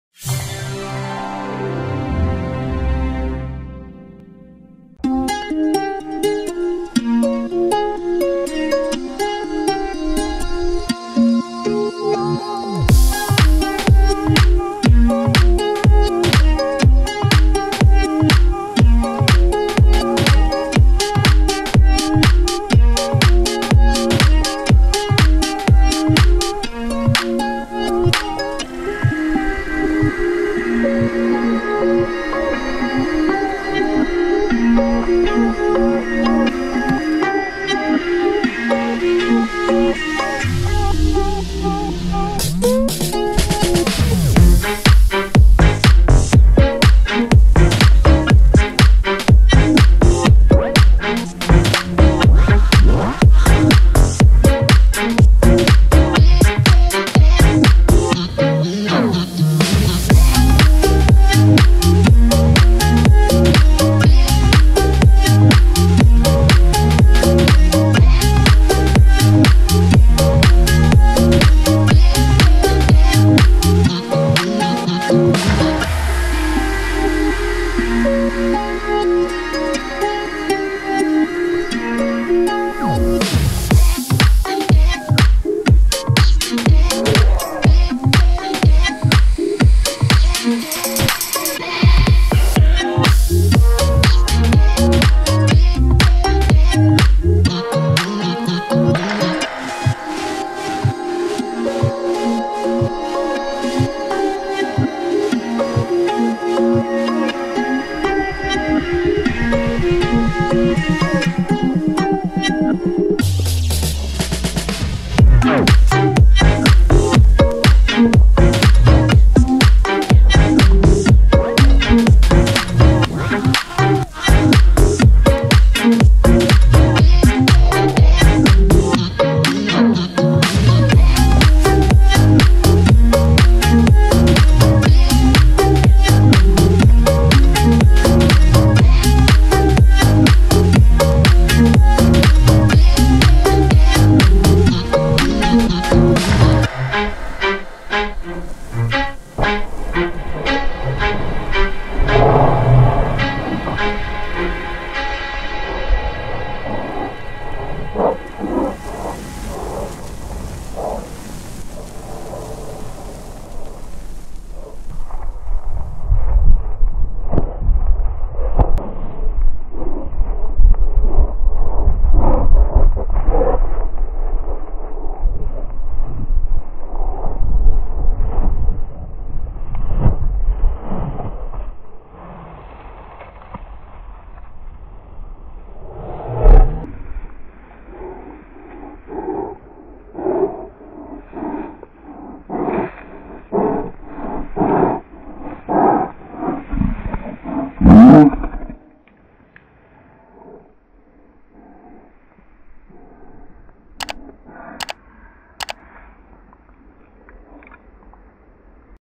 Cavalier King Charles Spaniel. Happy Puppy "BIRK" in slow motion.
The family has increased with a new member. A puppy!